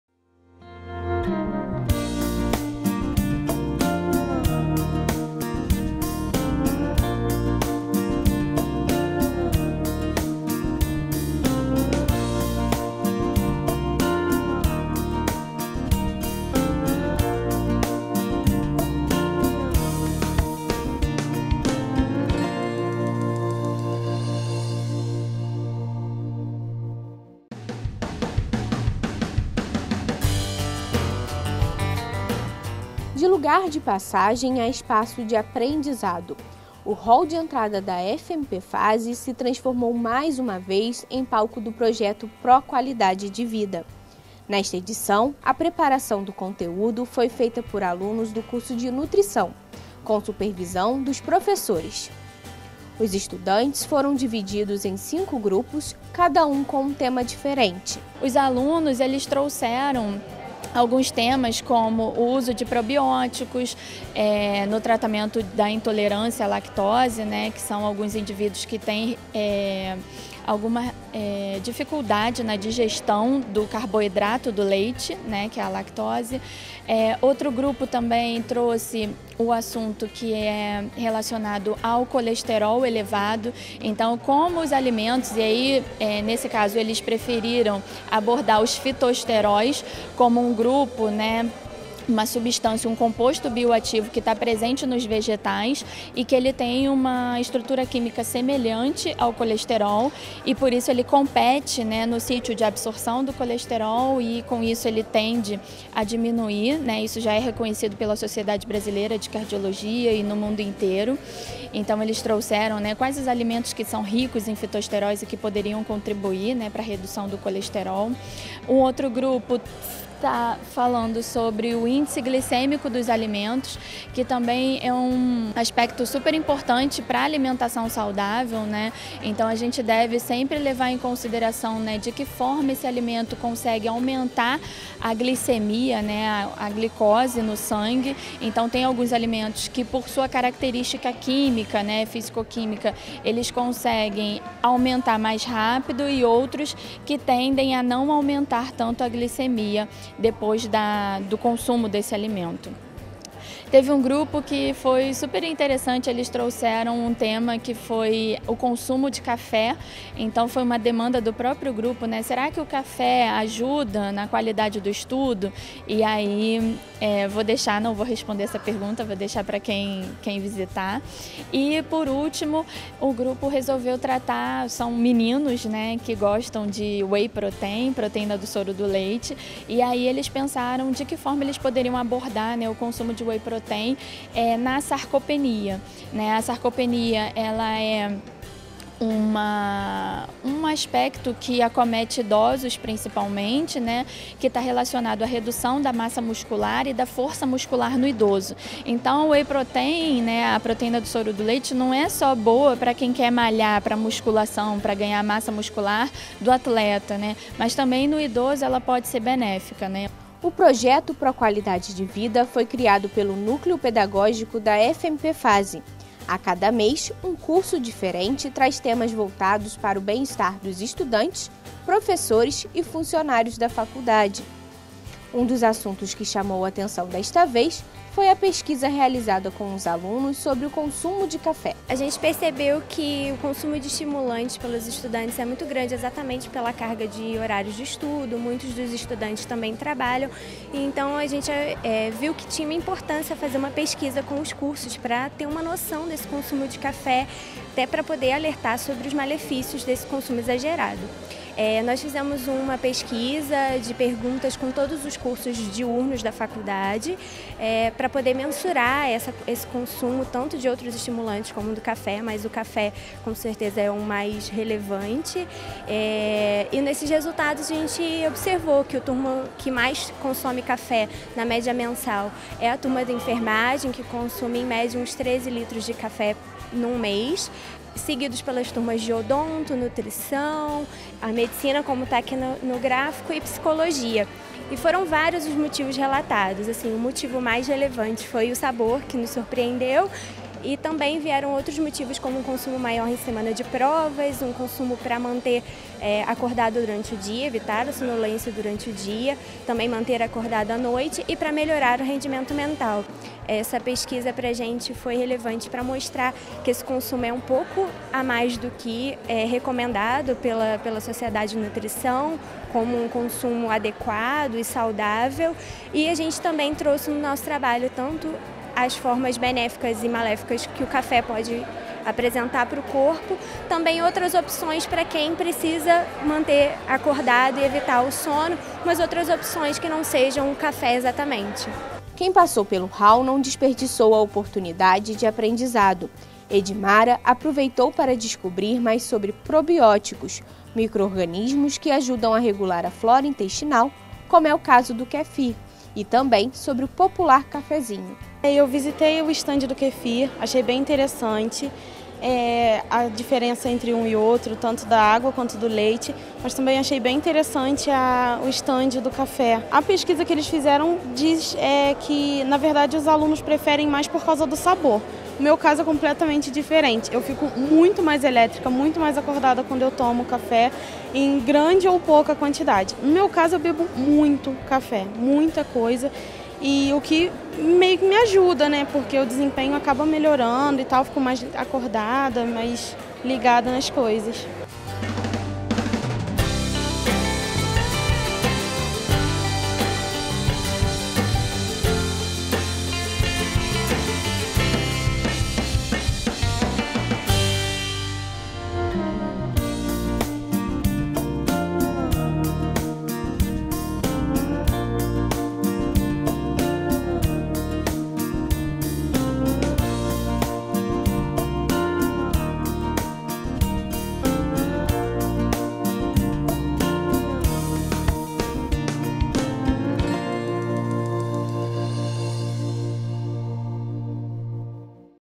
Minuto Fase: Pró - qualidade de Vida - Nutrição
O projeto Pró-Qualidade de Vida, criado pelo Núcleo Pedagógico da FMP/Fase, traz a cada mês um tema diferente voltado para o bem-estar dos estudantes, professores e funcionários da faculdade. Nesta edição, a preparação do conteúdo foi feita por alunos do curso de Nutrição.  Os estudantes foram divididos em cinco grupos, com temas como probióticos, whey protein (proteína do soro do leite) e o consumo de café como estimulante  pelos estudantes.